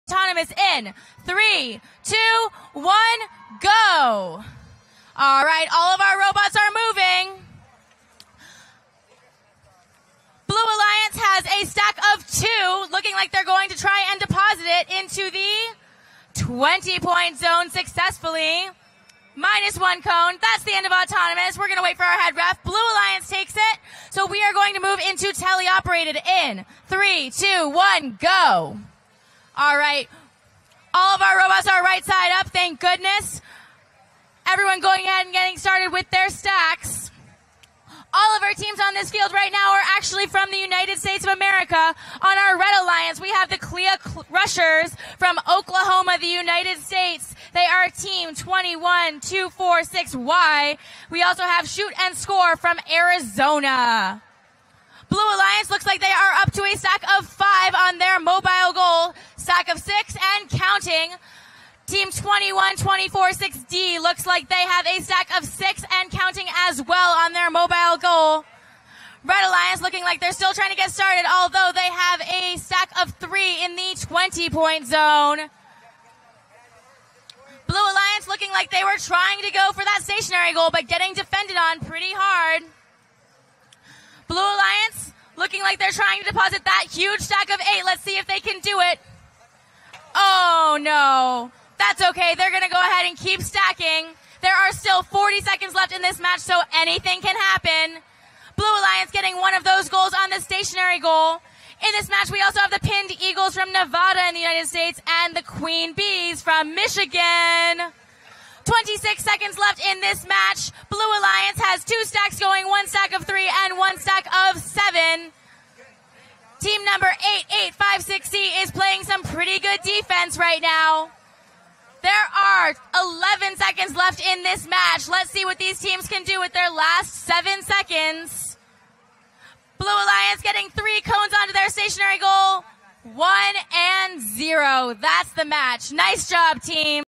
2018 VRC-MS Oppo Q169 - 8856C 68689A vs 21246D 8878D - 53 to 92
VEX Worlds 2018 - VRC Middle School - Opportunity Division - Qualifiers Match 169

Red - Score 53:
8856C (Cleora Crushers, Cleora Public School, Afton, Oklahoma, United States)
||  Opportunity Rank:52 . Wins:5 . Loses:5 . WP:10 . AP:20 . SP:556

68689A (Holland Bulldogs, Jerry D. Holland Middle School, Baldwin Park, California, United States)
||  Opportunity Rank:81 . Wins:2 . Loses:8 . WP:4 . AP:70 . SP:600

Blue - Score 92:
21246D (Dublin Robotics-Chargers, Dublin Robotics Club, Dublin, California, United States)
||  Opportunity Rank:2 . Wins:10 . Loses:0 . WP:20 . AP:100 . SP:637

8878D (Vexothermic Reaction, Jockey Hollow Robotics Team, Monroe, Connecticut, United States)
||  Opportunity Rank:17 . Wins:7 . Loses:3 . WP:14 . AP:50 . SP:633